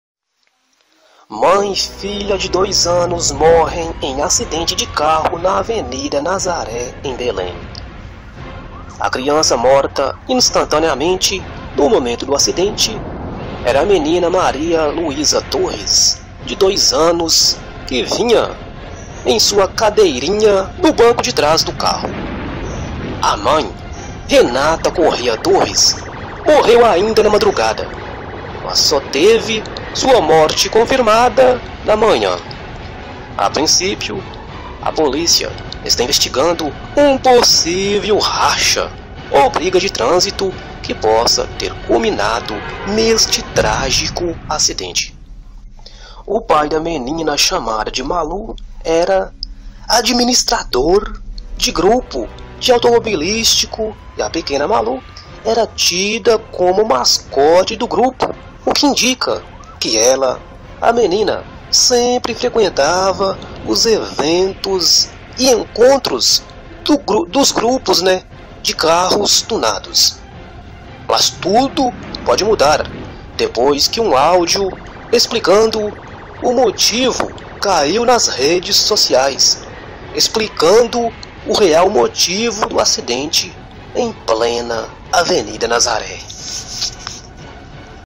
TRAGÉDIA! Carro DESTRUÍDO) MÃE e Filha de 02 Anos Morrem Em Violento Acidente de CARRO Em Belém
Tragédia CARRO Ficou Destruído! Mãe e Filha de 02 anos MORREM em Gravíssimo Acidente Em Belém.